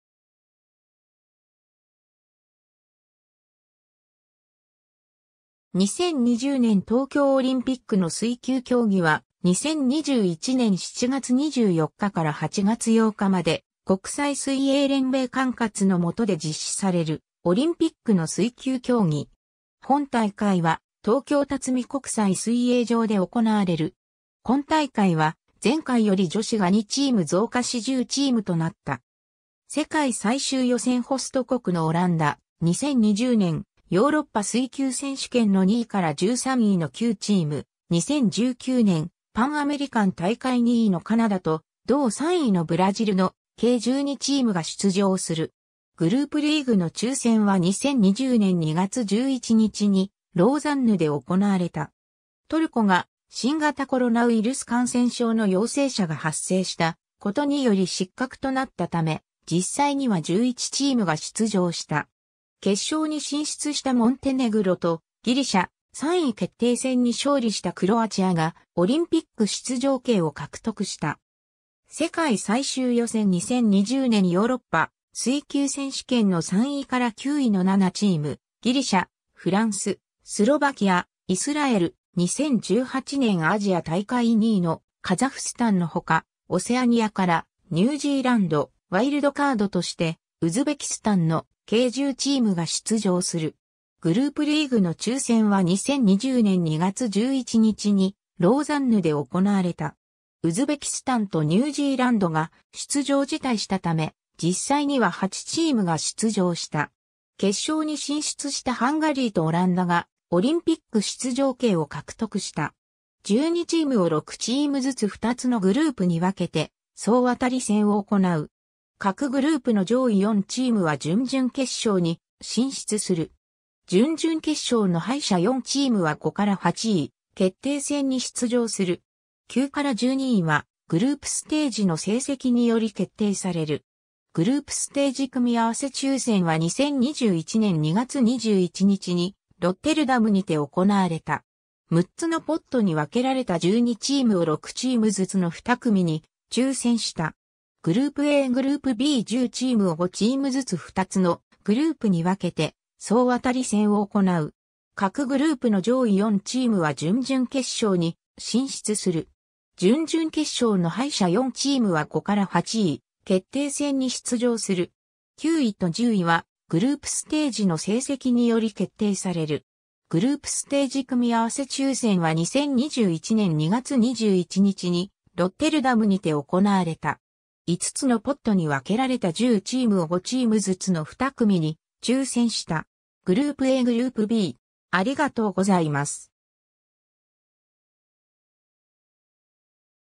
2020年東京オリンピックの水球競技
2020年東京オリンピックの水球競技（2020ねんとうきょうオリンピックのすいきゅうきょうぎ）は、2021年7月24日から8月8日まで、国際水泳連盟（FINA）管轄のもとで実施されるオリンピックの水球競技。
本大会は東京辰巳国際水泳場で行われる。
 今大会は前回より女子が2チーム増加し10チームとなった。
 世界最終予選 ホスト国のオランダ、2020年ヨーロッパ水球選手権の2位～13位（既に出場権を得ている、2位：スペイン、5位：セルビア、6位：イタリアを除く）の9チーム（モンテネグロ、クロアチア、ギリシャ、ロシア、ドイツ、ジョージア、ルーマニア、トルコ、フランス）、2019年パンアメリカン大会2位のカナダと同3位のブラジルの計12チームが出場する。
（パンアメリカン大会4位のアルゼンチンと2018年アジア大会上位のイラン・中国が辞退したため、代わりにヨーロッパのチームが出場する。） グループリーグの抽選は2020年2月11日に、ローザンヌで行われた。
トルコが新型コロナウイルス感染症の陽性者が発生したことにより失格となったため、実際には11チームが出場した。
決勝に進出したモンテネグロとギリシャ、3位決定戦に勝利したクロアチアがオリンピック出場権を獲得した。
 世界最終予選 2020年ヨーロッパ水球選手権の3位～9位の7チーム（ハンガリー、オランダ、イタリア（ホスト国）、ギリシャ、フランス、スロバキア、イスラエル）、2018年アジア大会2位のカザフスタンのほか、オセアニアからニュージーランド、ワイルドカードとしてウズベキスタン（アジア大会不出場）の計10チームが出場する。
 グループリーグの抽選は2020年2月11日に、ローザンヌで行われた。
ウズベキスタンとニュージーランドが出場辞退したため、実際には8チームが出場した。
決勝に進出したハンガリーとオランダがオリンピック出場権を獲得した。
 12チームを6チームずつ2つのグループに分けて総当り戦を行う。
各グループの上位4チームは準々決勝に進出する。
準々決勝の敗者4チームは5-8位決定戦に出場する。
9-12位はグループステージの成績により決定される。
 グループステージ 組み合わせ抽選は2021年2月21日にロッテルダムにて行われた。
6つのポットに分けられた12チームを6チームずつの2組に抽選した。
 グループA グループB 10チームを5チームずつ2つのグループに分けて総当り戦を行う。
9位と10位はグループステージの成績により決定される。
5つのポットに分けられた10チームを5チームずつの2組に抽選した。
 グループA グループB